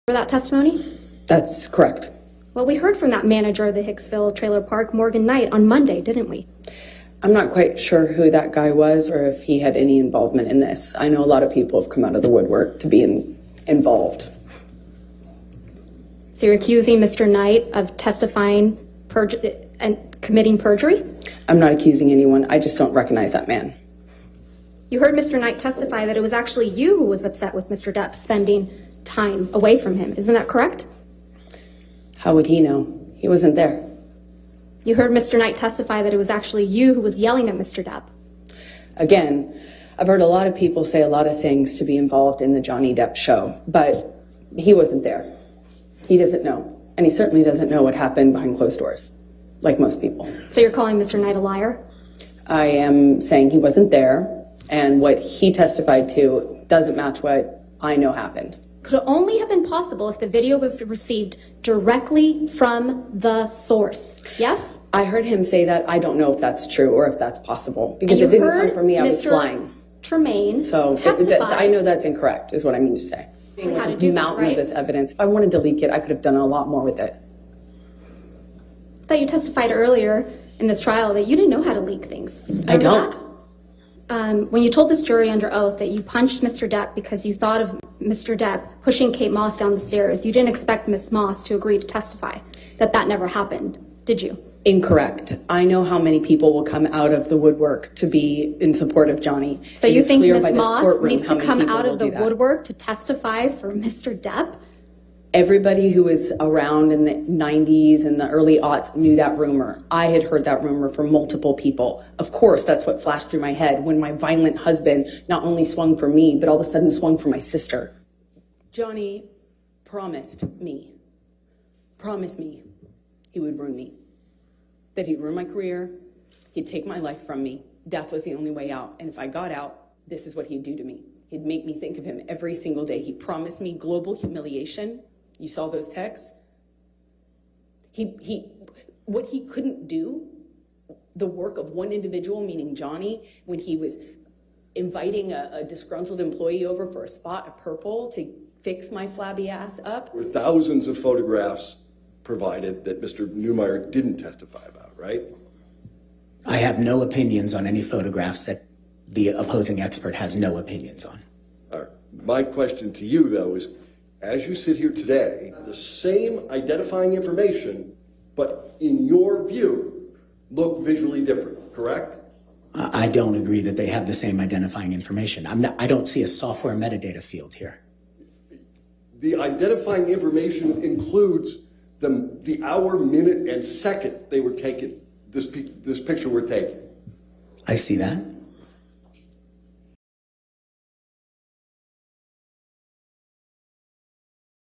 Compilation of Ben Chew's Funny Reaction's (pt. 2)
We set up a small compilation of Ben Chew and his funny facial expressions during court. We feel exactly the same, Ben 😂

Heres a few steps to help us out:

2. Like

We look forward to see you in the comments at our next video! 😊